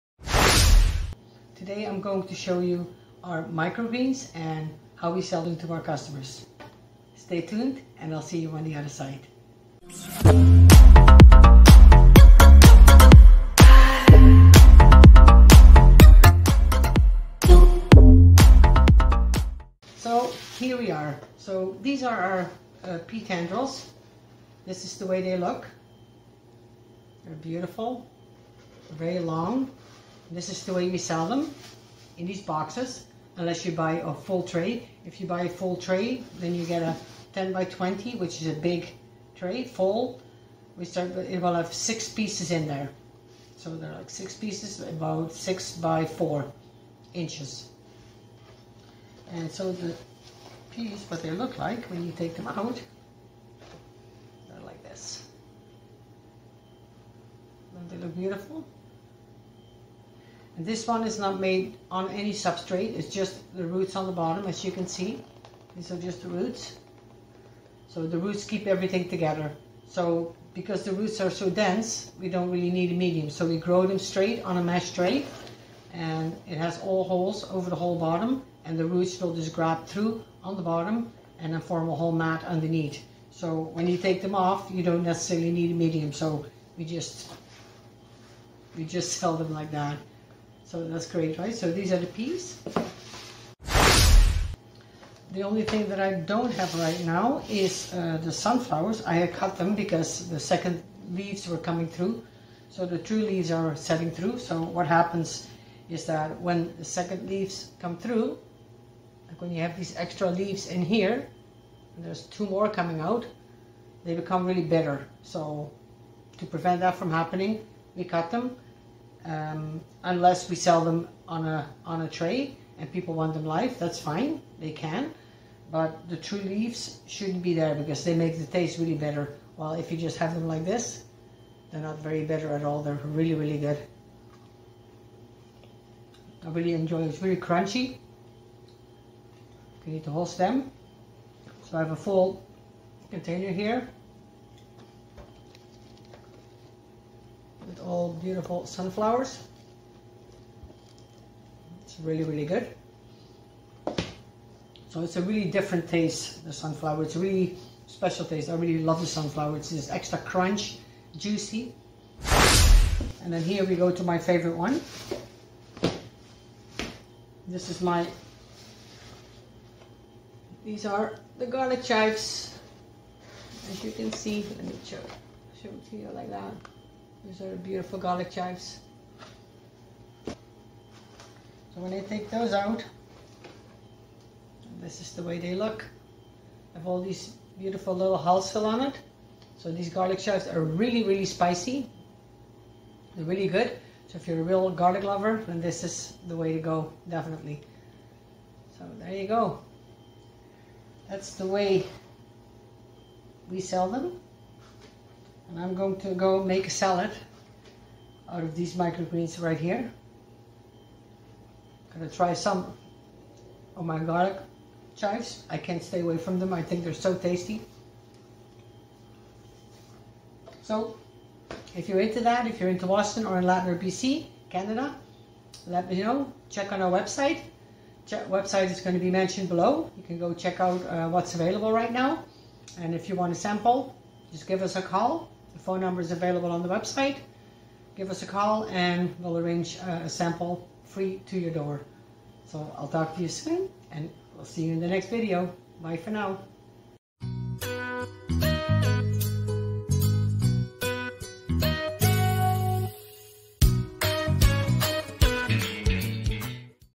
How We Package Live Microgreens | scMicroGro
🔶In this episode i'll show you some of our microgreens 🌱 and how we package Live microgreens for our clients.

🔷I'll give some specifics on the Live Micro-Sunflower, Micro-Peas and Micro-Garlic Chives. You will see up close what these microgreens look like. Click the play button and enjoy.